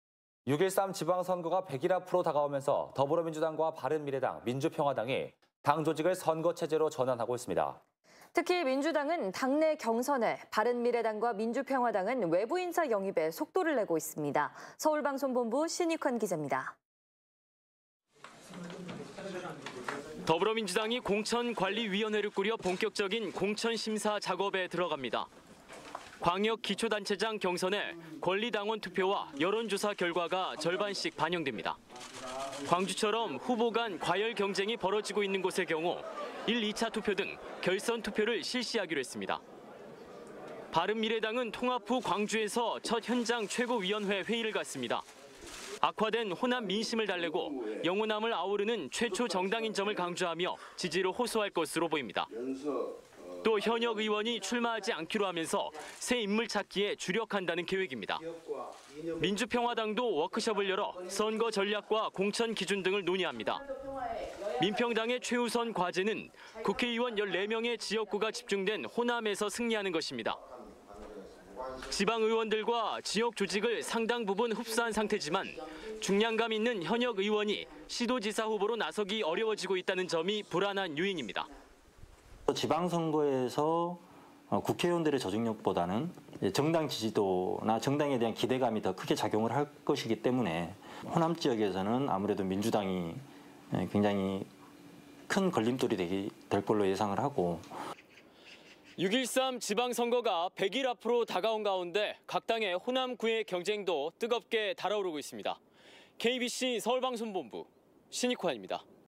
지방선거 D-100..'호남 잡아라'
6.13 지방선거가 100일 앞으로 다가오면서 더불어민주당과 바른미래당, 민주평화당이 당 조직을 선거체제로 전환하고 있습니다.

특히 민주당은 당 내 경선을, 바른미래당과 민주평화당은 외부인사 영입에 속도를 내고 있습니다.

서울방송본부 신익환 기잡니다.